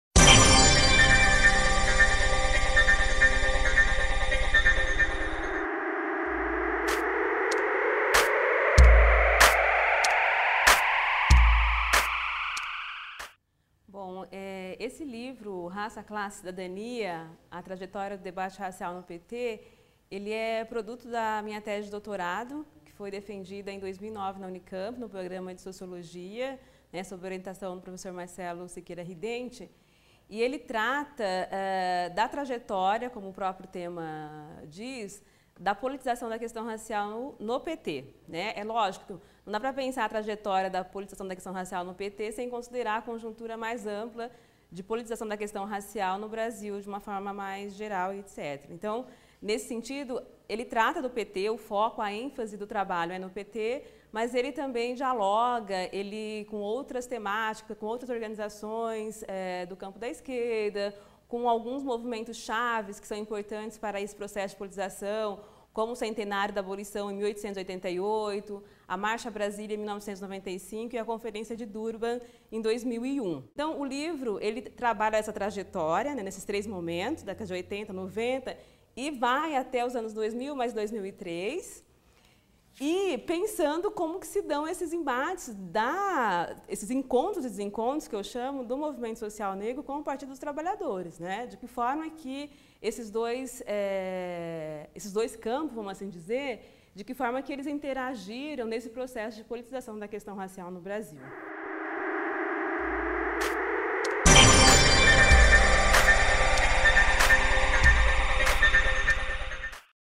Entrevista com Claudete Gomes Soares
Claudete Gomes Soares é professora da Universidade Federal da Fronteira Sul na cidade de Chapecó-SC. Nesta entrevista exclusiva para a TevêFPA, ela fala sobre os pilares que compõem a estrutura do seu livro Raça, Classe e cidadania:  a trajetoria do debate racial no Partido dos Trabalhadores (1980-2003).